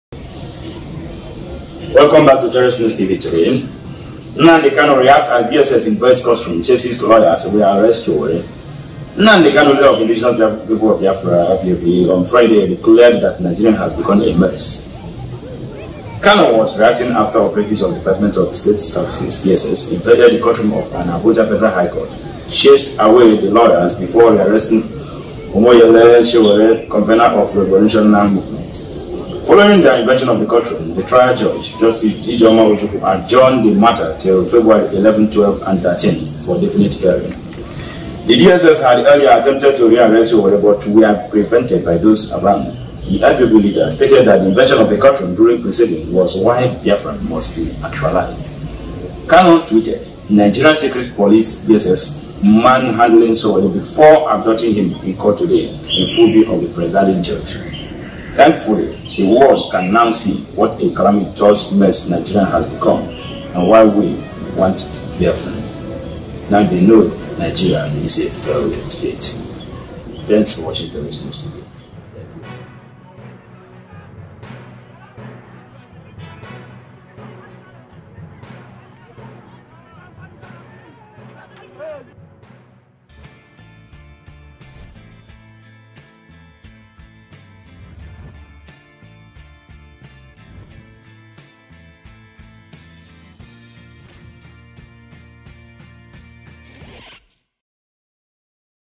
Sowore rearrested - Nnamdi Kanu reacts as DSS invades courtroom & chases lawyers
Kanu tweeted: “Nigeria’s secret police DSS manhandling Sowore before abducting him in court today in full view of the presiding judge.
“Thankfully the world can now see what a calamitous mess Nigeria has become and why we want Biafra. Now they know Nigeria is a failed state.